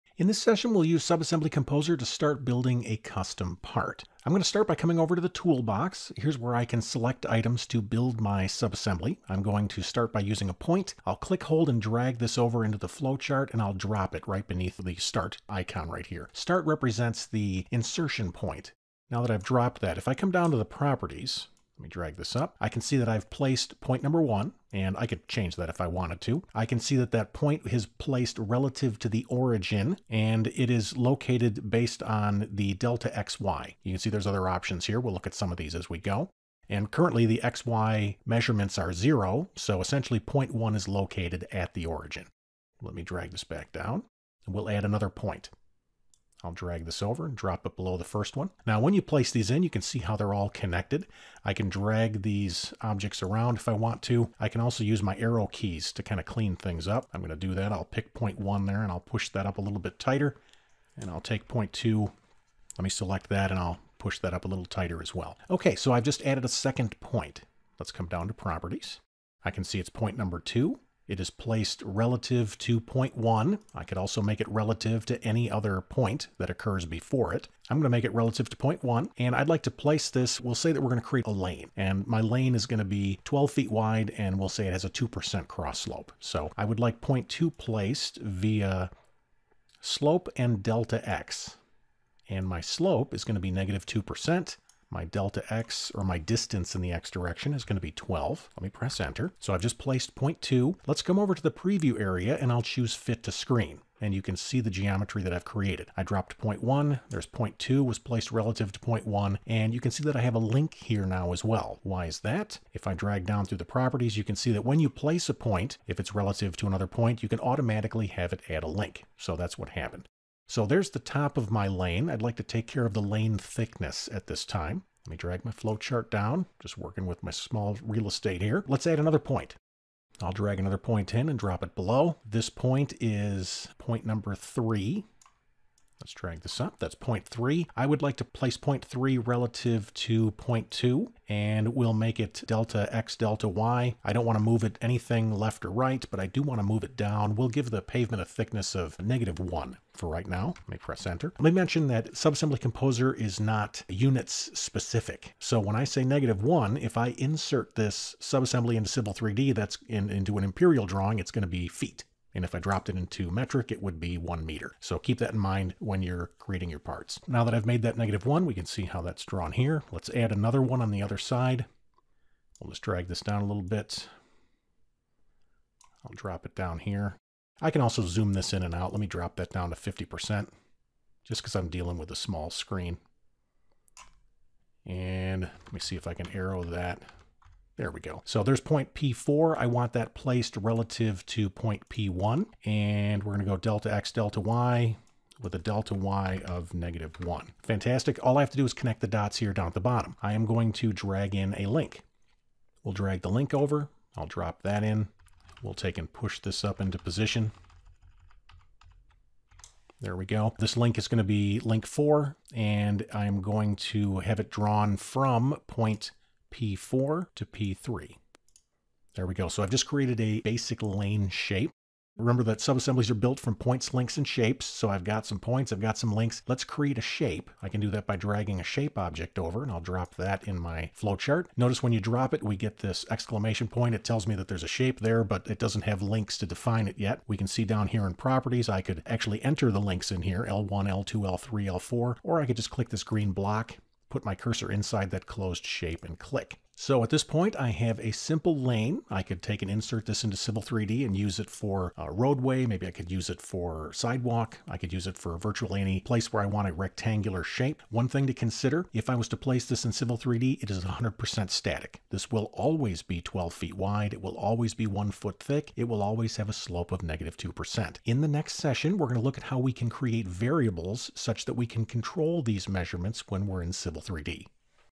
Subassembly Composer Pt. 2 - Creating Parts Using Points Links & Shapes
This recording is part of a "quick start" series designed to help you get up and running using Subassembly Composer.  Subassembly Composer is an application that is included with your Civil 3D installation.  Using Subassembly Composer you can easily create custom subassembly parts for use in a Civil 3D corridor model.  In this session we'll learn how to create parts using points links and shapes.